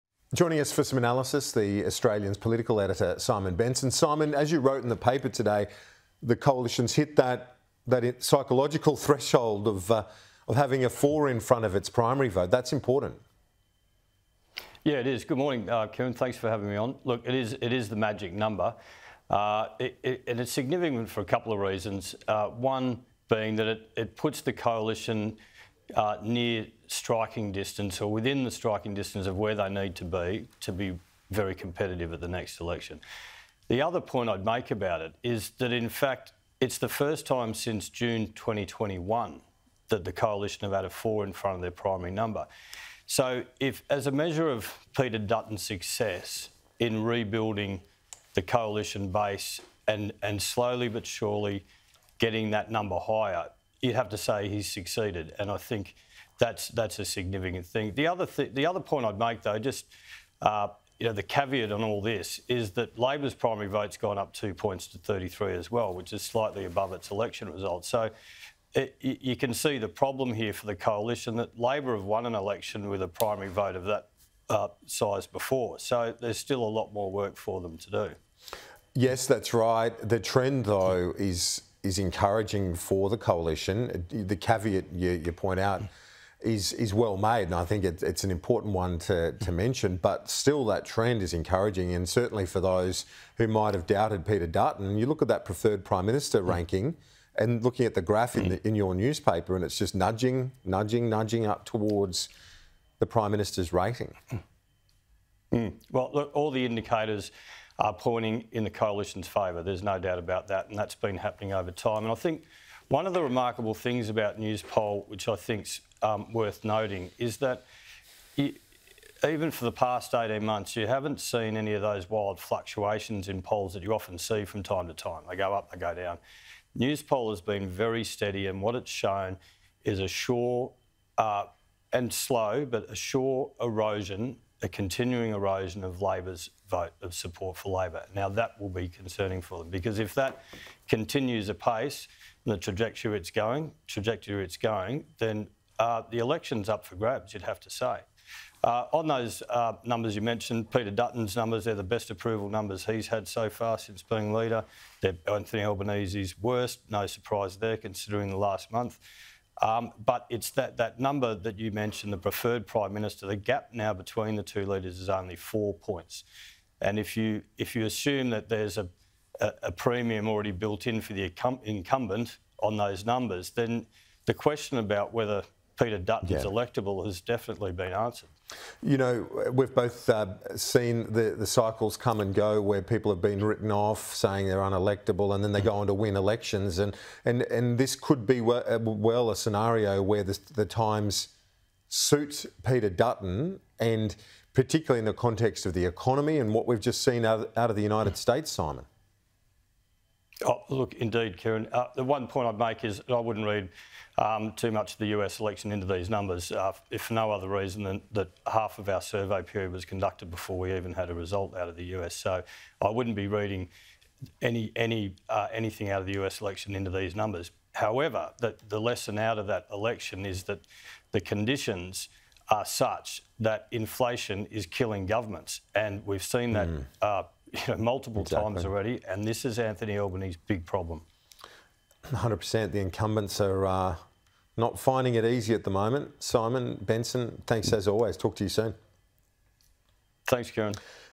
Coalition ‘within striking distance’ of being ‘very competitive’ at next election
The Australian's Political Editor Simon Benson says the latest Newspoll indicates the Coalition is “within striking distance” of where they need to be to be “very competitive” at the next federal election.

Support for the Coalition has increased to its highest level since the 2022 election, according to the latest Newspoll.

“It puts the Coalition within striking distance, or near striking distance of where they need to be to be very competitive at the next election,” Mr Benson told Sky News Australia.

“The other point I’d make about it is, in fact, it’s the first time since June 2021 that the Coalition have had a four in front of their primary number.”